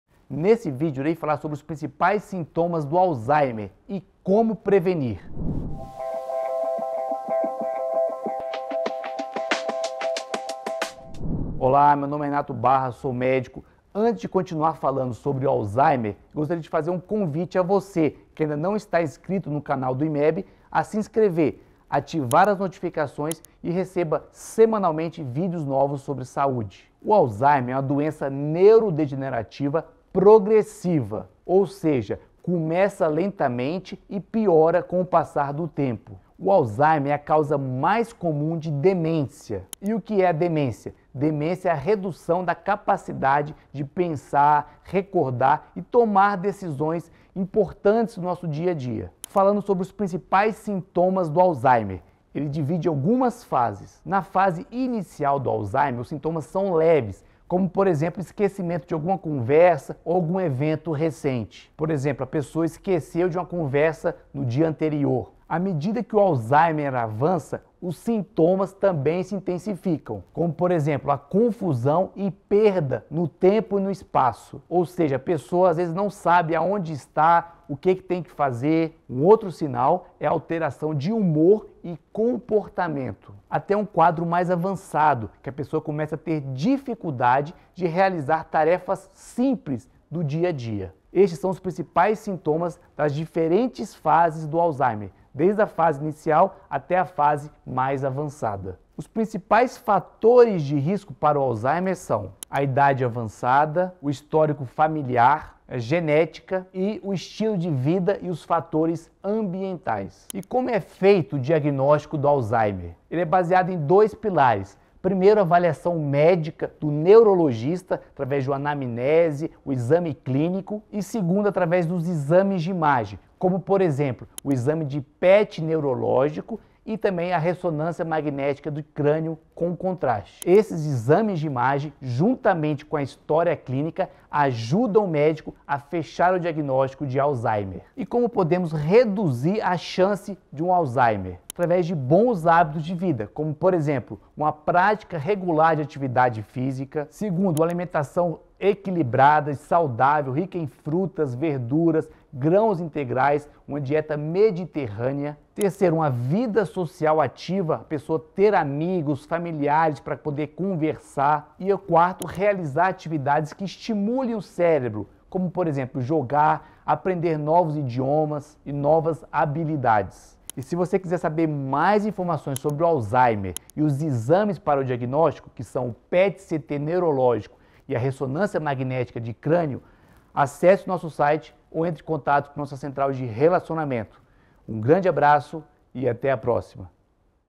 Sintomas do Alzheimer: Diagnóstico e Fatores de Risco
Os sintomas do Alzheimer podem variar desde leves esquecimentos até a incapacidade de realizar tarefas simples. Neste vídeo, o Dr. Renato Barra explica detalhadamente os sinais do Alzheimer e como é feito o diagnóstico dessa condição que é a causa mais comum de demência.

O que você vai aprender neste vídeo:

🔸 Sintomas do Alzheimer: Compreenda as diferentes fases do Alzheimer, desde os esquecimentos leves na fase inicial até a confusão e alteração de humor nas fases mais avançadas.

🔸 Fatores de Risco: Conheça os principais fatores de risco para o desenvolvimento do Alzheimer, incluindo idade avançada, histórico familiar, genética, estilo de vida e fatores ambientais.

🔸 Diagnóstico: Saiba como é realizado o diagnóstico do Alzheimer através da avaliação médica, anamnese, exame clínico e exames de imagem como PET neurológico e ressonância magnética do crânio.

🔸 Prevenção e Bons Hábitos: Descubra como bons hábitos de vida podem reduzir a chance de desenvolver Alzheimer. Prática regular de atividade física, alimentação equilibrada, vida social ativa e atividades que estimulem o cérebro são fundamentais.

Para mais informações sobre o Alzheimer e os exames necessários para o diagnóstico, assista ao vídeo completo. Visite nosso site ou entre em contato com nossa central de relacionamento para mais detalhes.